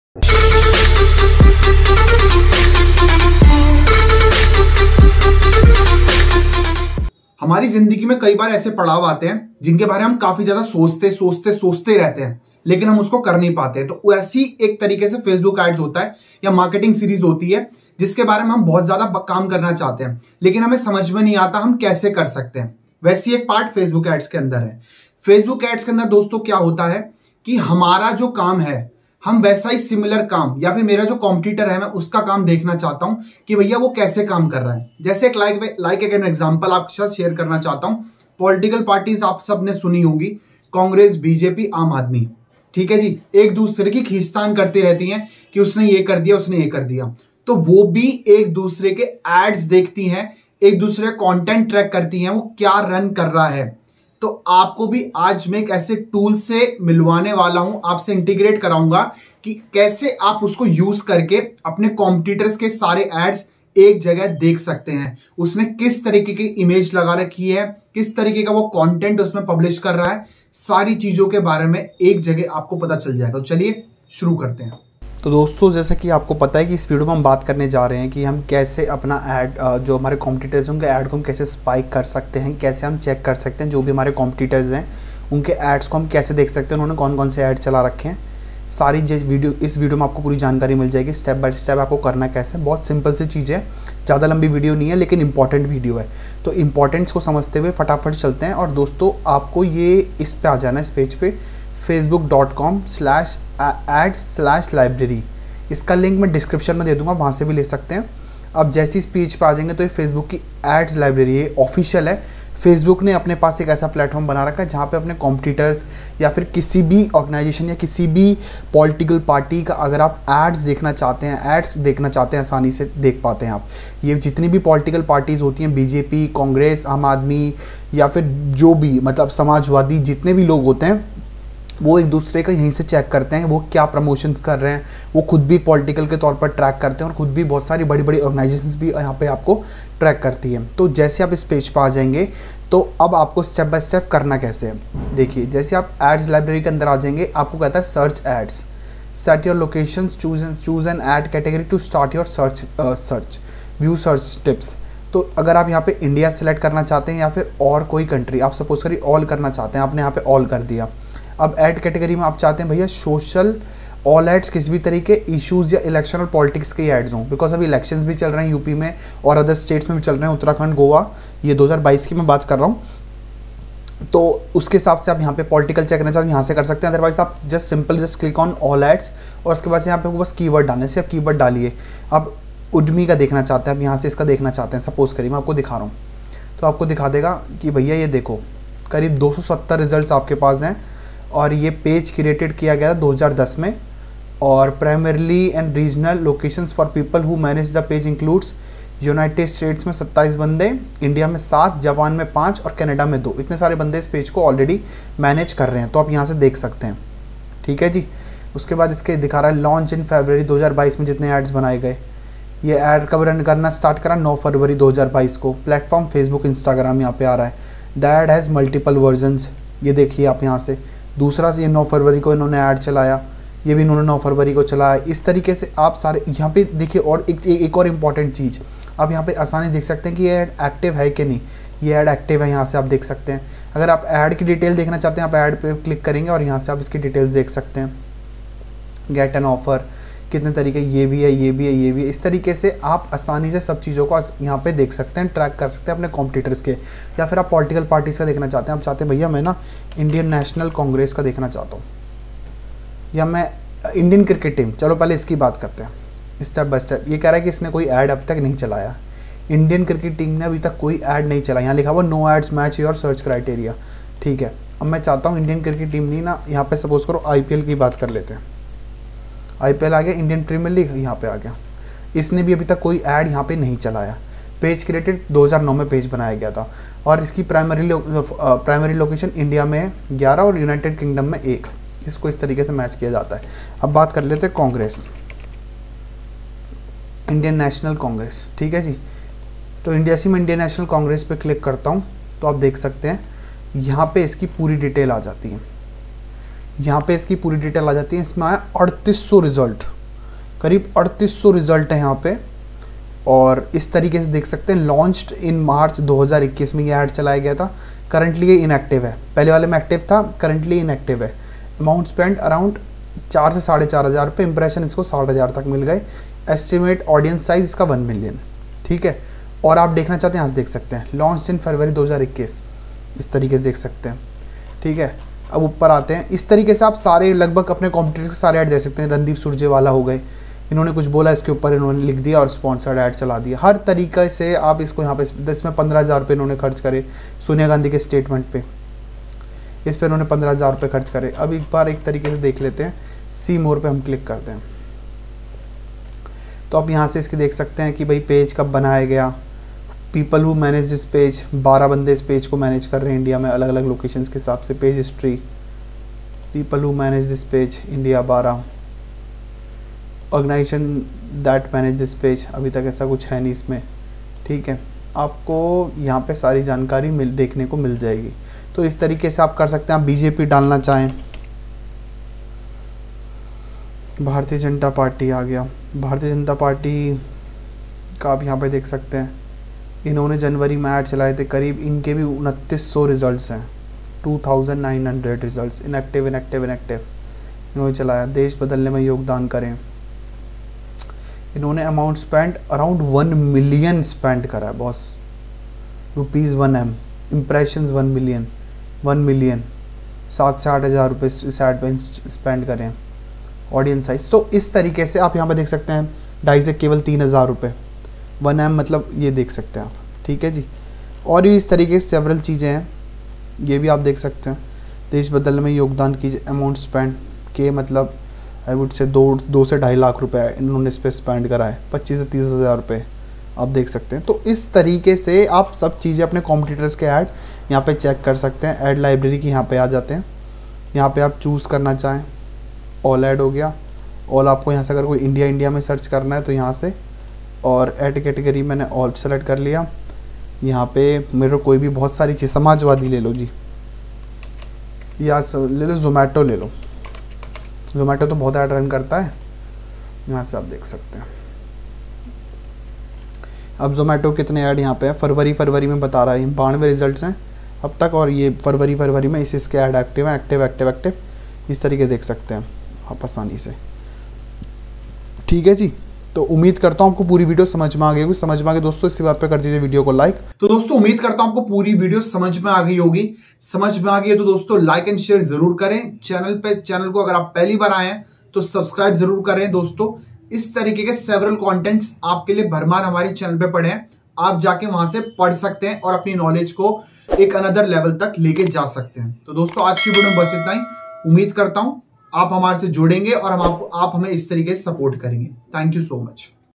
How to Spy Your Competitor's Facebook Ads For FREE | Spy on Your Competitors Ads 2022 | Ravi Sharma
In this video, We are explaining about How to Spy Your Competitor's Facebook Ads For FREE. Please do watch the complete video for in-depth information.